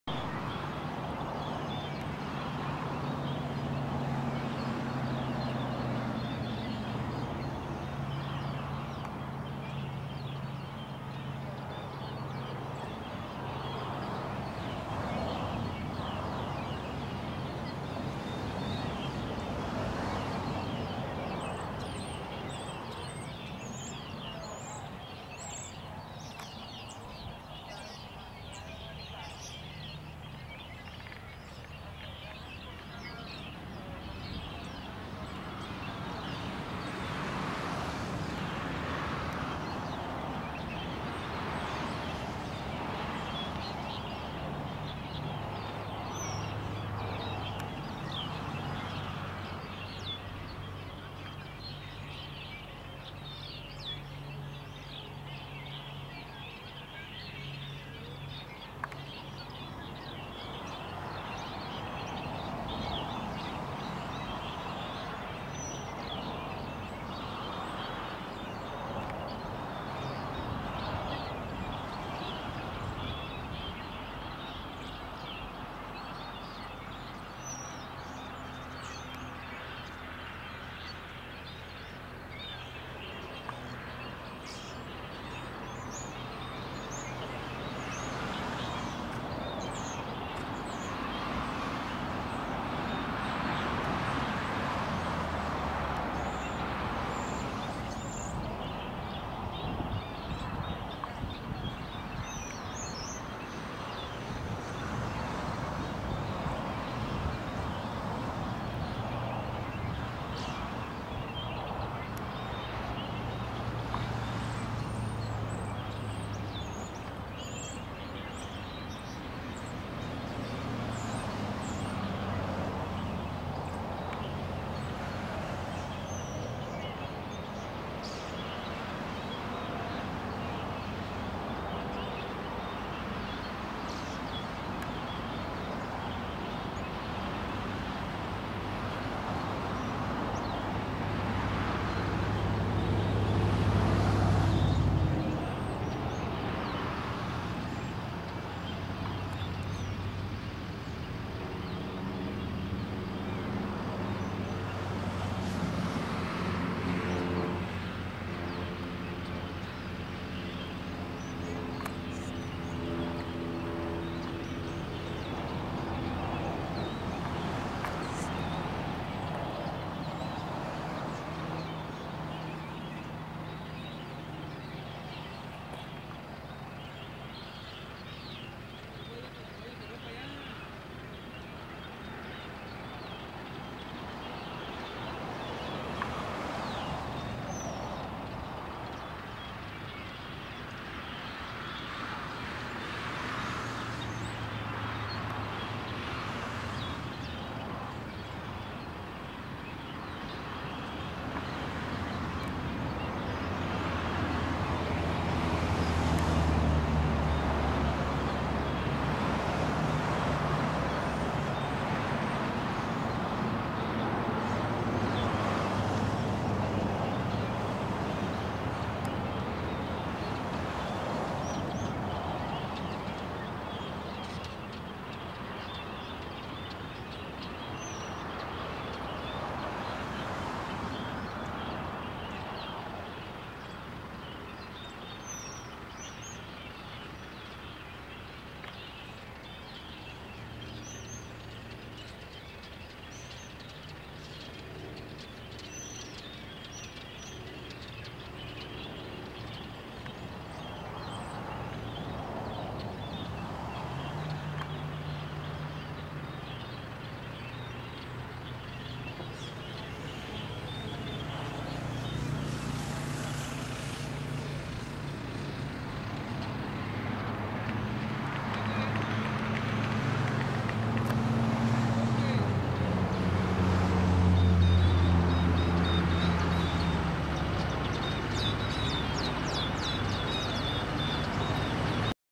Catalina - CDS Annual Championships 2011
Catalina - ridden October 2011 at Rancho Murieta, California. CDS Annual Championships